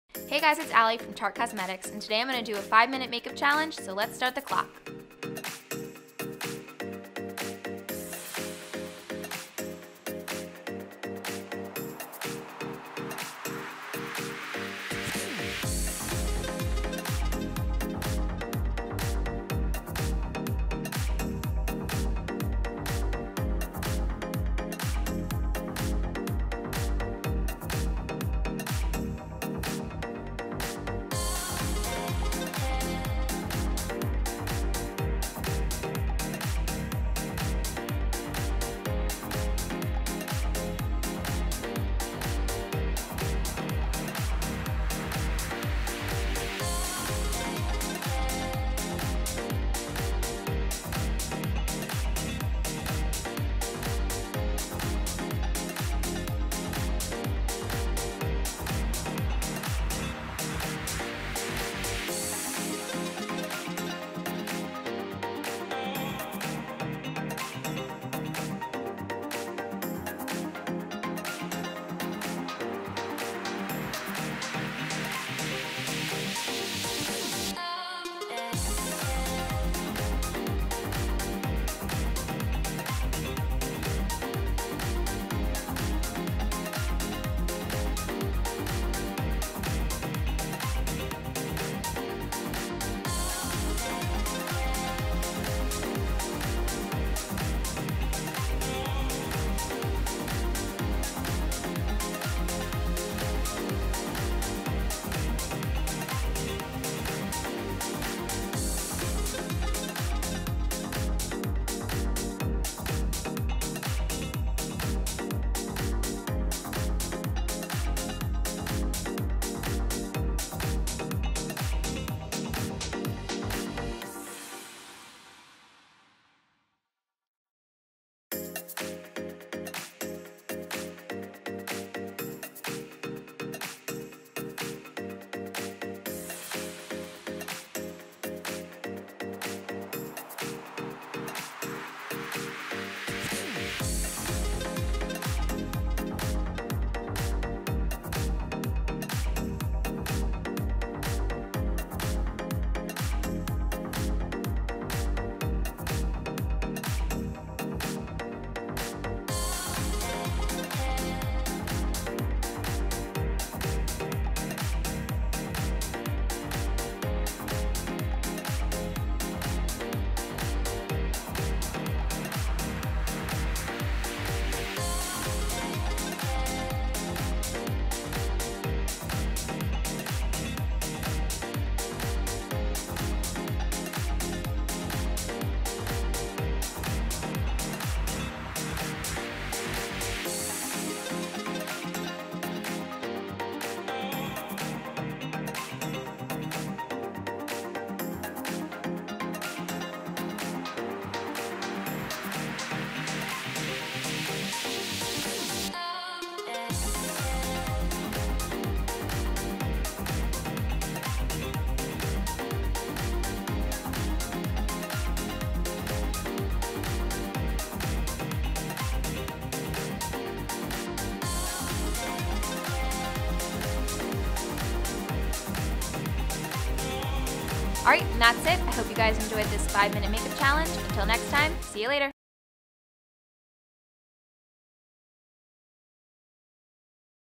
How to Nail a Full Makeup Look in Under 5-Minutes
Ally from Tarte Cosmetics takes the challenge and completes her full look in just under 5-minutes. Watch the challenge then take it yourself, sharing online with MacysBeauty.

Find everything you need to look & feel your best at Macy’s: The Beauty Destination.